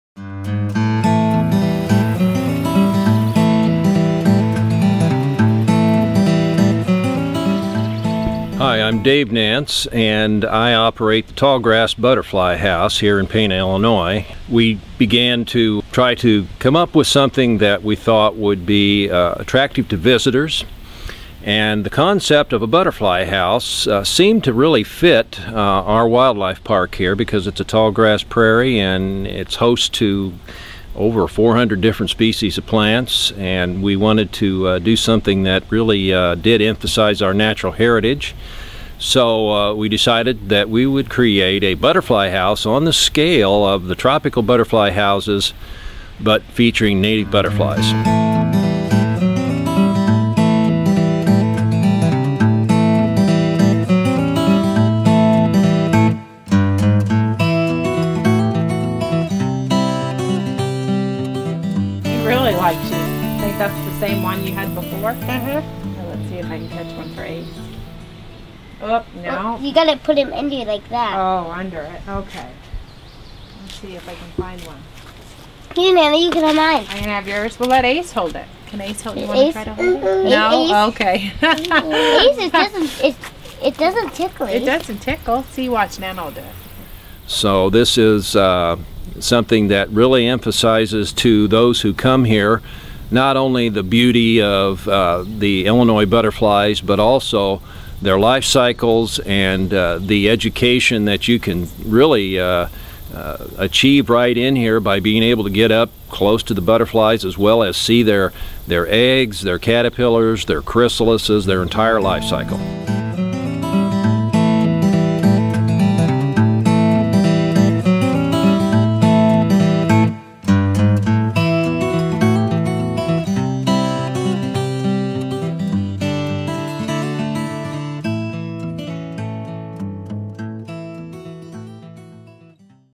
Tallgrass Butterfly House
The Tallgrass Butterfly House in Pana, Illinois is a showcase for native butterflies.